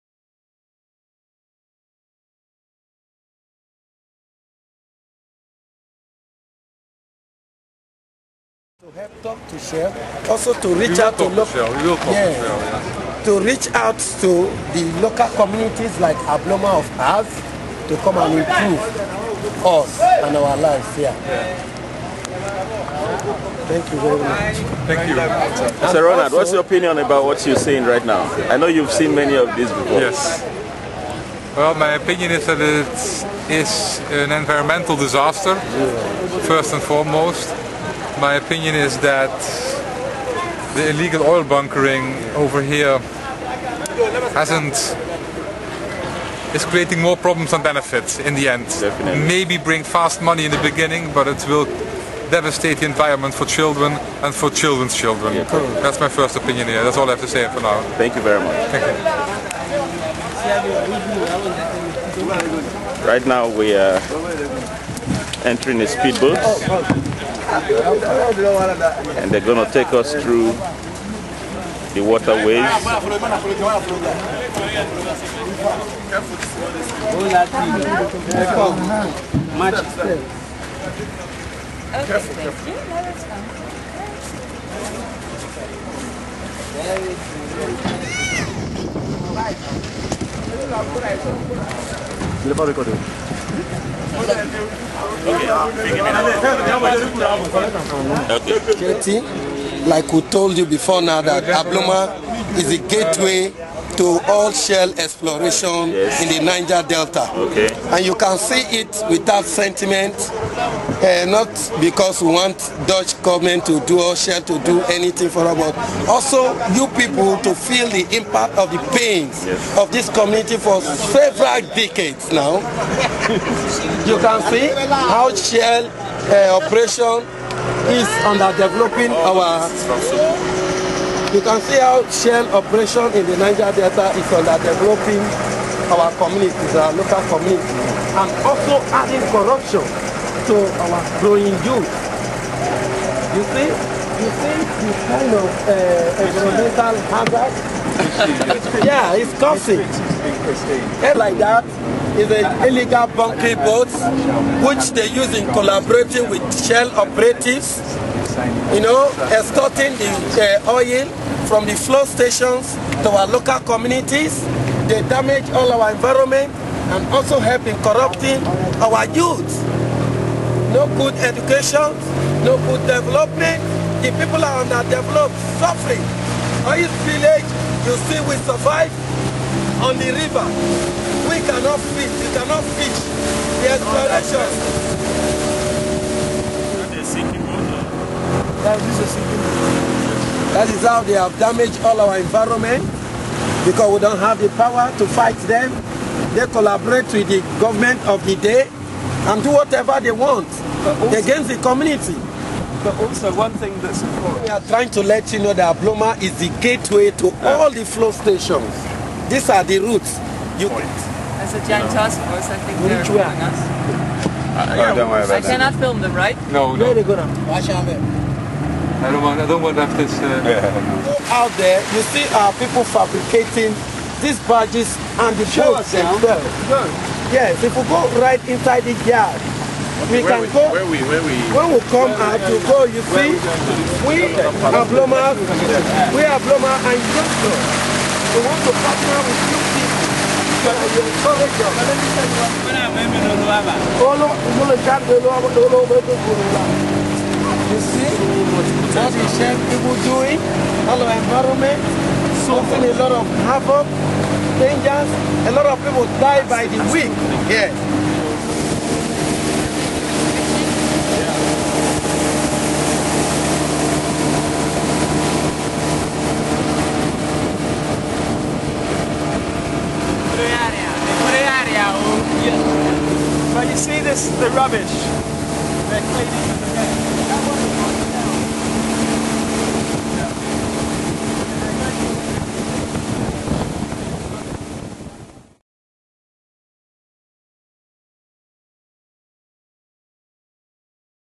Abuloma Oil Bunkering
Comrade Sunny Ofehe visits the Niger Delta with Sharon Gesthuizen, Dutch Member of Parliament. Here the are being informed about the Oil Bunkering in Abuloma